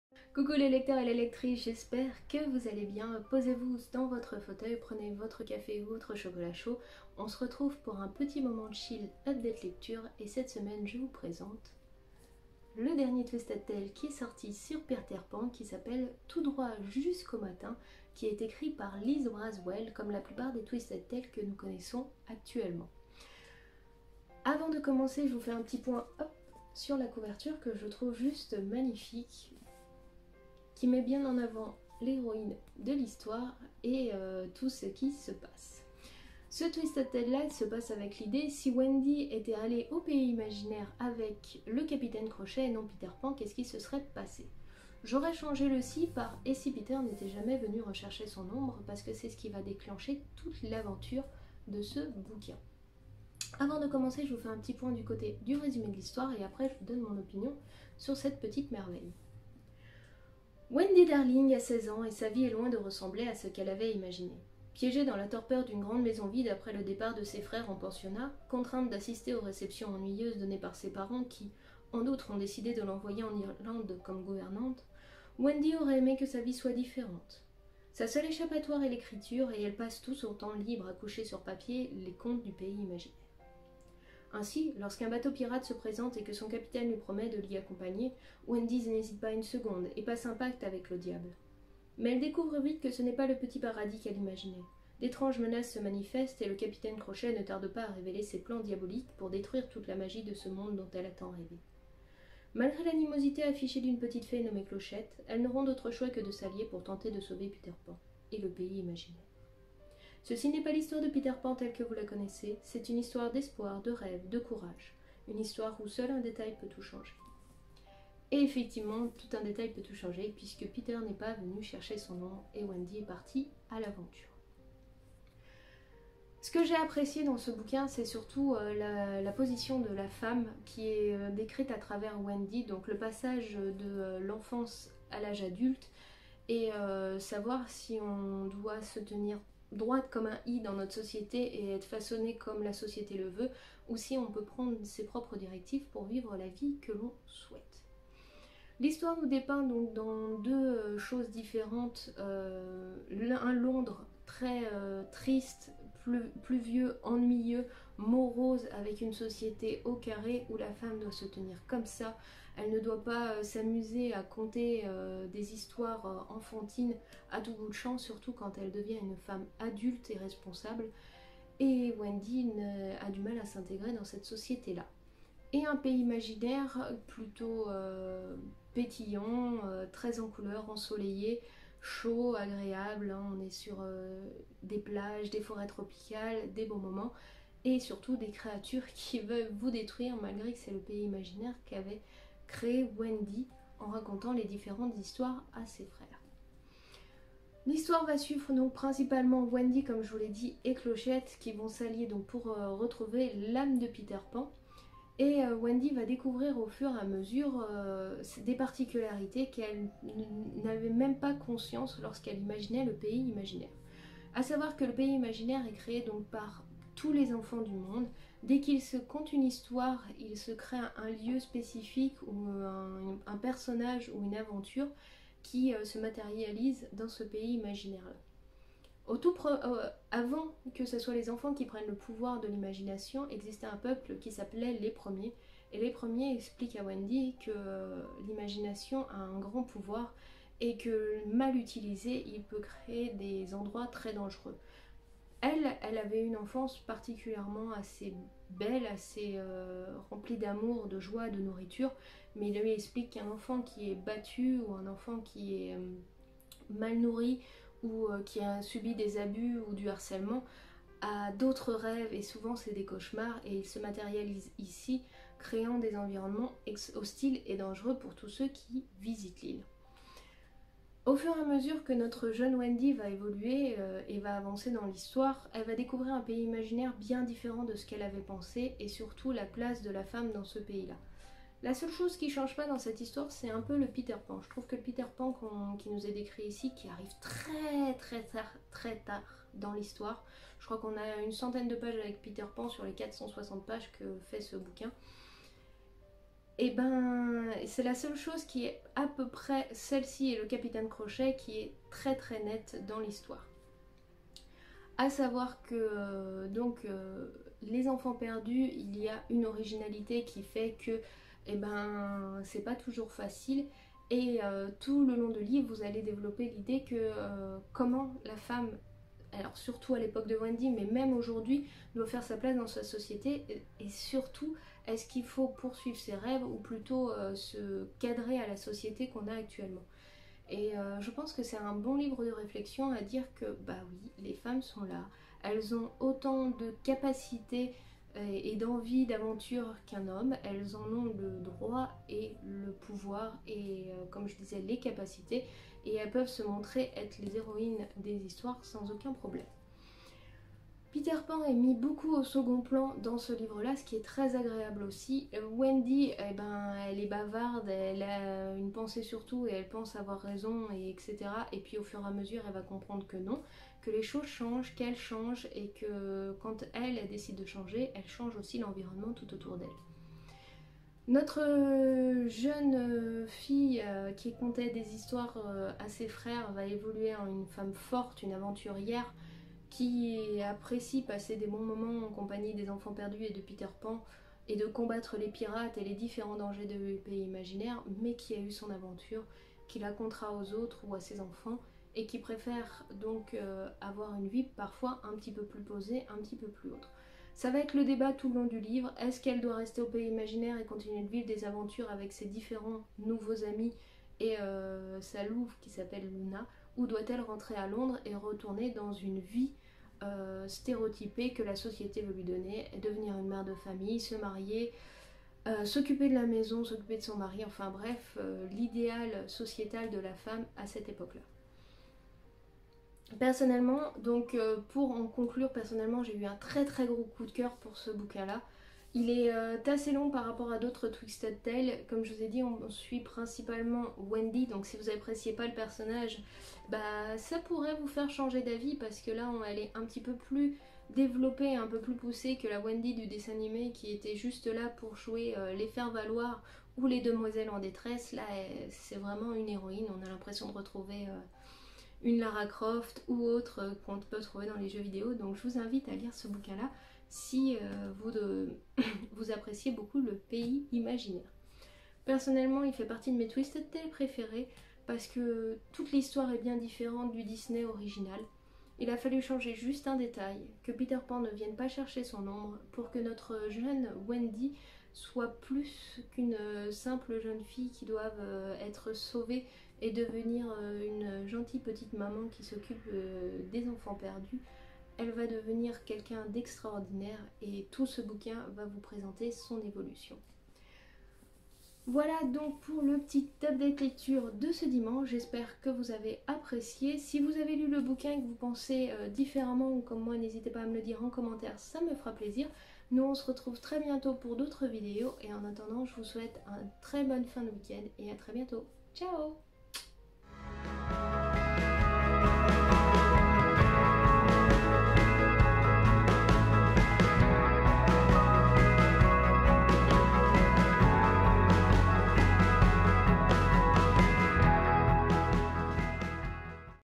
Twisted Tale: Tout droit jusqu'au matin, une aventure au pays imaginaire.
Coucou les bibliomanes, aujourd'hui on fait un point lecture sur un Twisted tale sorti il y'a peu qui n'est autre que Tout droit jusqu'au matin de Liz Braswell aux éditions Hachette Heroes Disney.
Venez lire les aventures de la jeune lady du nom de Wendy Moira Angela Darling qui est loin d’être la femme en détresse, éperdument amoureuse de Peter Pan, que l'on connaissait dans le dessin animé de Disney de 1953.
Voici l'histoire de femmes indépendantes, fortes et prêtent a tout pour sauver ce qui leur tient le plus a cœur, sans devoir attendre l'arrivée du prince charmant sur son beau cheval blanc.

Bonne découverte a vous et a bientôt